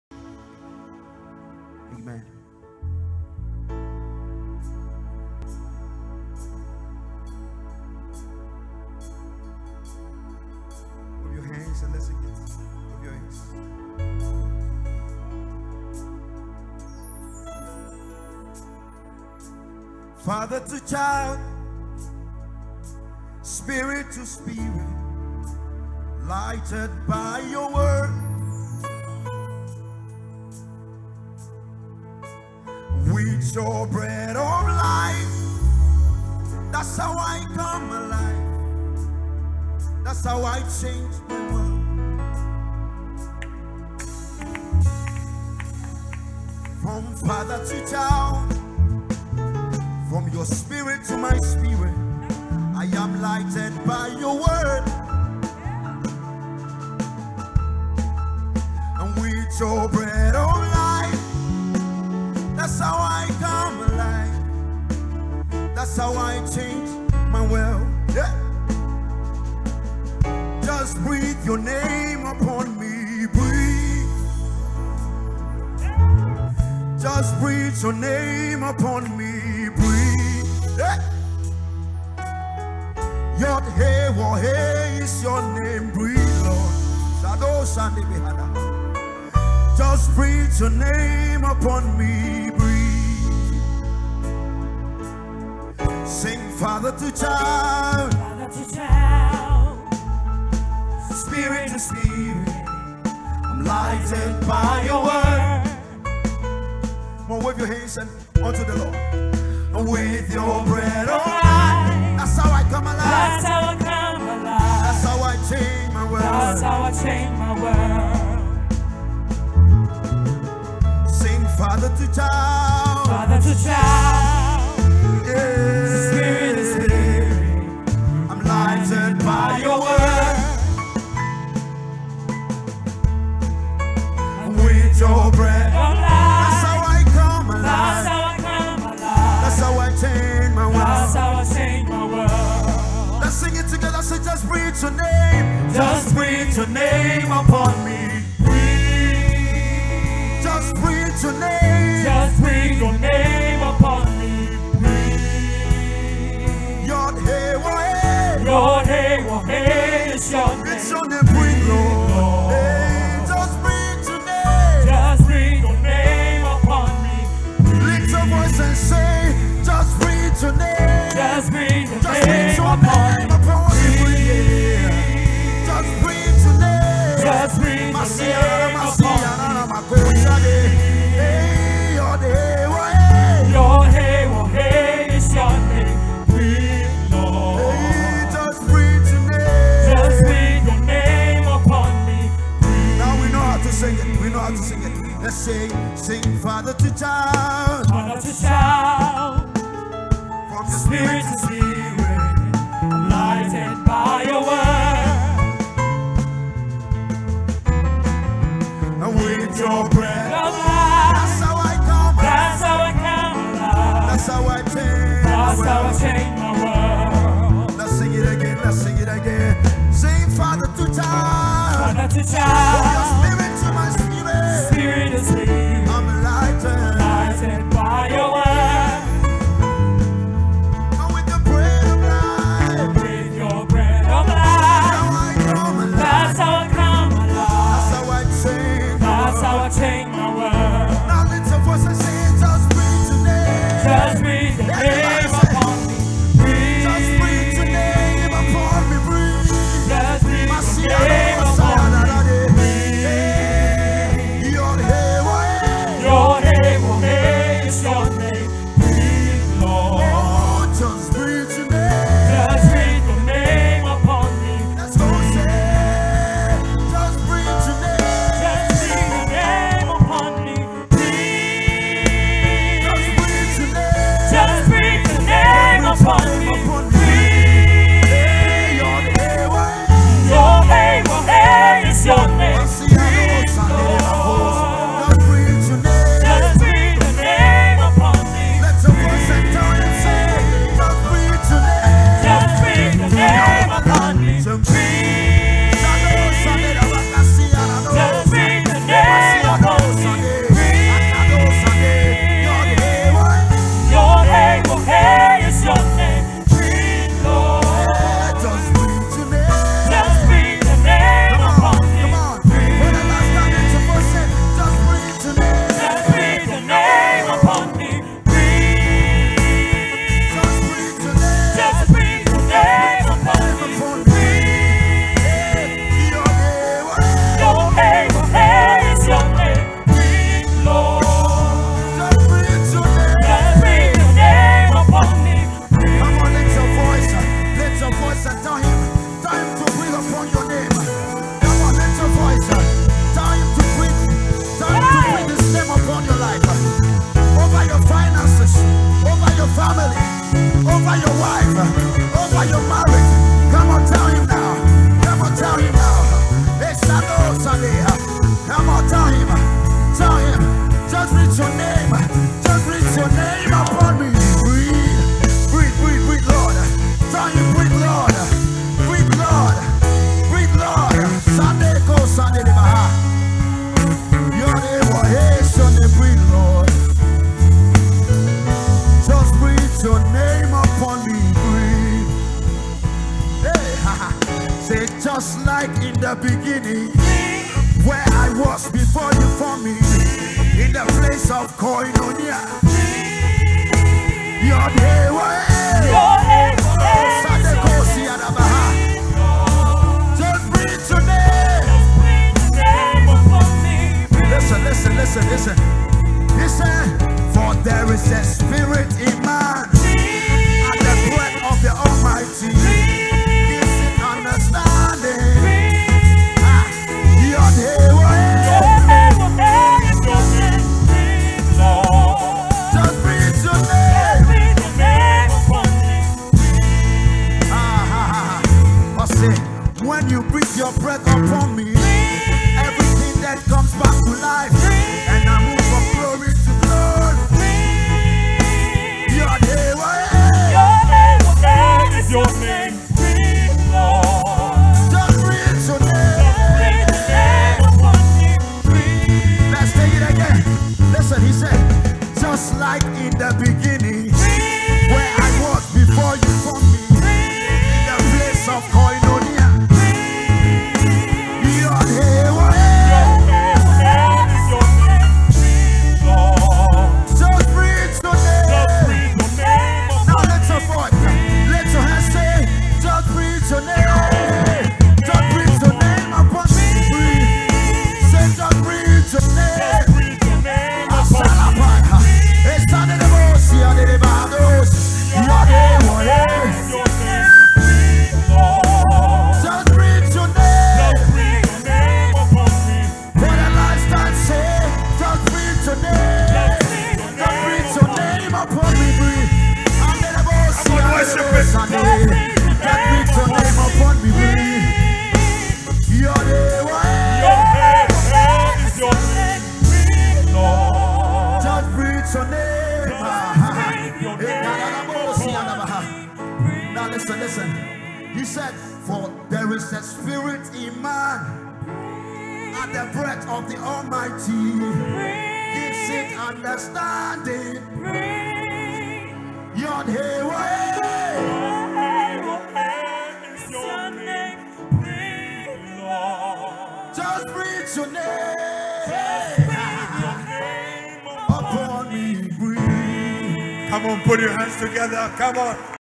Song Ministration - Holy Ghost Encounter Service (October)
Song ministration during our Holy Ghost Encounter Service

04/10/2020

Venue: Holiness Temple - Adenta - Frafraha, Mile 13 Junction(Off the Dodowa Road)